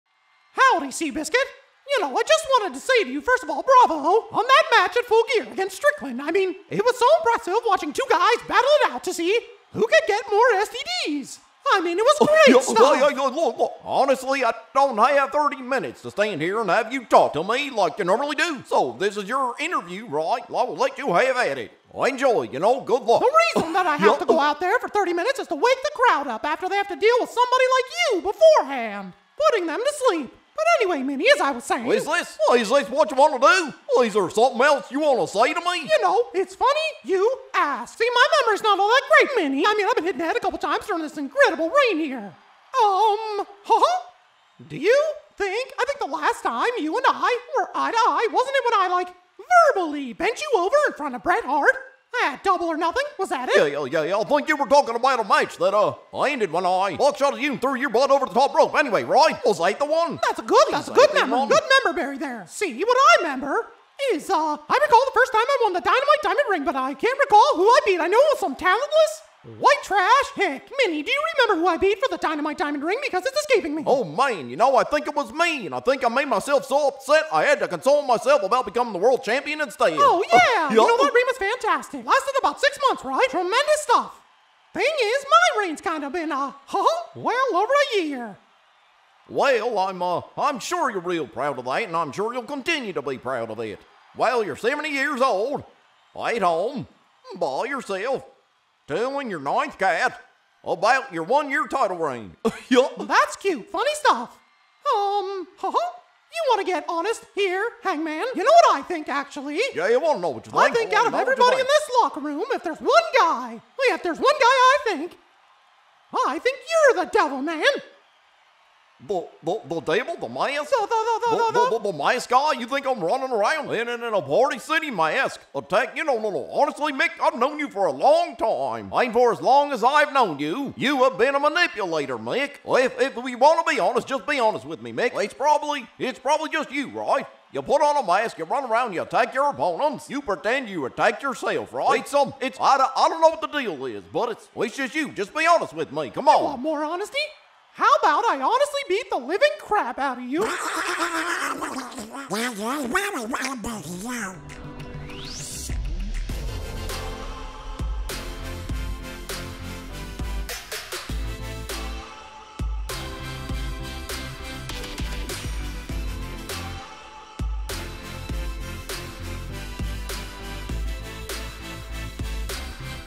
AEW Hangman Adam Page & MJF Promo, but it's Mickey & Goofy
Another wrestling voice dub with a request of Hangman Adam Page and MJF as Mickey and Goofy.